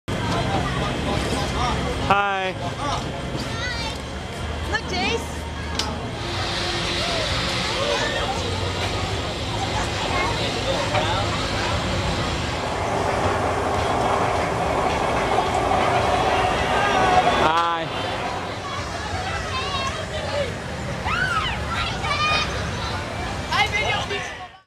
Young Friends
My kids and their young friends while we were at worlds of fun in Kansas City